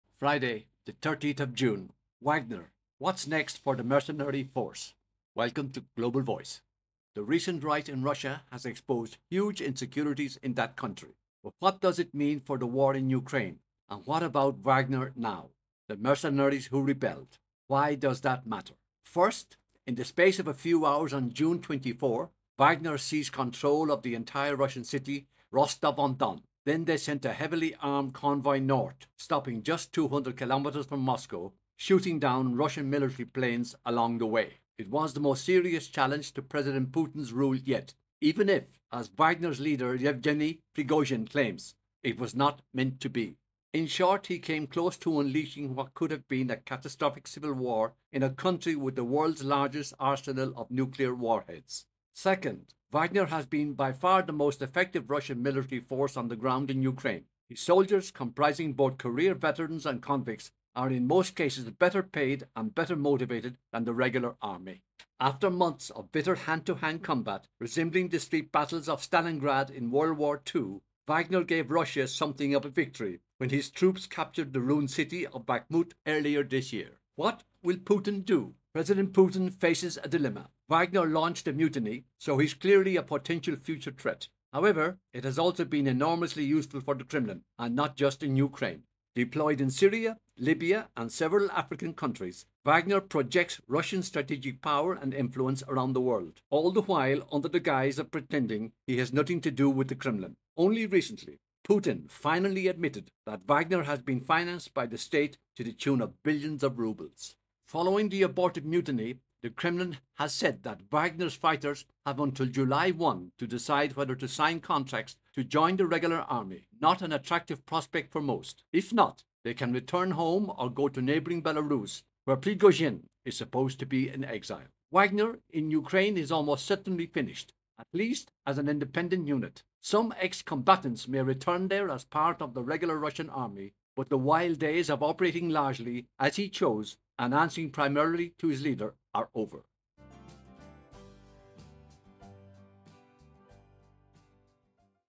Wagner: What's next for the mercenary force?
ºWhere is Prigozhin?

We know from flight tracking data that a plane linked to Mr. Prigozhin landed in the Belarusian capital Minsk this week. His arrival there was confirmed by the leader of Belarus, Alexander Lukashenko.
But that same plane has since returned to Russia, with stops in St. Petersburg and Moscow.
But where is he now? Will he be allowed to continue running Wagner's operations from Belarus, despite his betrayal of his former patron, President Putin?

He could still be useful to Russia, but Putin is unlikely to trust him or forgive him for coming close to unleashing a civil war. There is a grim precedent for revenge being brought to bear on those believed to have betrayed the state.

British investigators concluded that former KGB officers turned defectors, Alexander Litvinienko and Sergei Skripal, were poisoned in Britain by agents sent by Moscow. Prigozhin will now look over his shoulder for the rest of his life.

"Russia is effectively disbanding one of its most combat-capable assault forces," says Kateryna Stepanenko, a Russia analyst at the Institute for the Study of Warfare. "That's a big deal for Ukrainian fighters who have in the past pointed out that fighting Wagner was much worse than fighting regular Russian units."
The signs are that the future of Wagner's group is still being discussed in negotiations between Prigozhin, the Kremlin and Lukashenko, who has offered Wagner's troops the use of a military camp.

Satellite images appear to show activity at a disused base about 13 miles (21 km) from the town of Asipovichy, some 64 miles from the capital Minsk. The area has been reported in the Russian media as a place that could house Wagner fighters.

Ukraine now has to worry about reinforcing its northern border, while neighboring NATO countries Poland, Latvia and Lithuania have concerns about future subversive activities Wagner might launch from Belarus.

And yet no one, probably not even the Kremlin itself, knows how many Wagner fighters will go to Belarus, how many will join the Russian army to fight in Ukraine, and how many will hang up their boots and go home.

A scour of online Telegram chat rooms by BBC Verify reveals few clues, though a blogger who identifies himself as a Belarusian Wagner fighter says the group is "still working". Immediately after the mutiny, many are believed to have returned to their bases in the Russian-occupied parts of eastern Ukraine.

What impact on the conflict?

BBC Verify has been looking for evidence of Ukrainian forces' advances since Wagner's rebellion, but has so far not observed any significant change in frontline positions.
"I don't think it will have an impact at the moment, as Russia is on the defensive," says Marina Miron, a Russian military expert at King's College London.

"If the Russian regular army manages to absorb Wagner's troops and reorient them under a single command structure, it will not necessarily weaken the Russian side."

Ultimately, it comes down to a question of morale among the Russian front-line troops. If they suspect their commanders are fighting among themselves in Russia, that could seriously affect morale in the trenches over time.

Wagner isn't done, but the days of operating him largely independently of the Russian military are over. With his weapons delivered and without the charismatic Prigozhin at the helm, he will no longer be the force that he was. And that, for kyiv, is a relief.